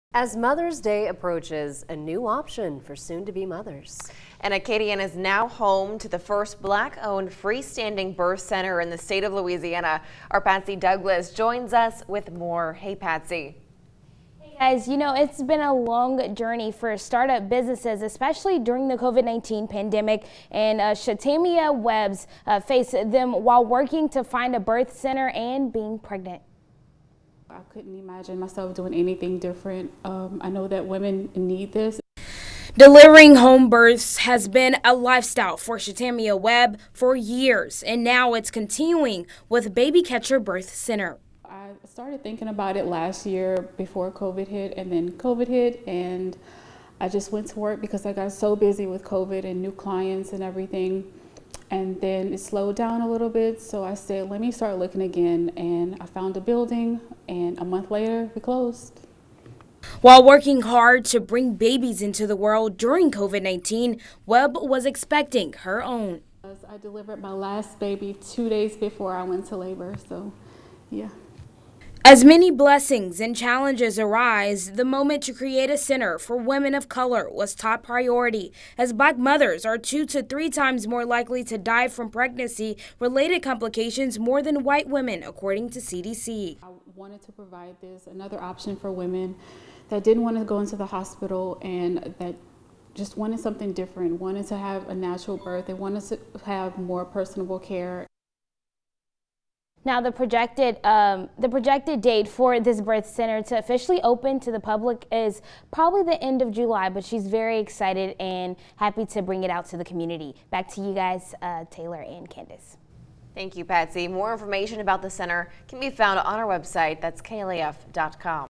Lafayette Black Owned Birth Center
A new option for soon to be mothers is coming to Acadiana. The first owned freestanding birth center is now here in the state of Louisiana.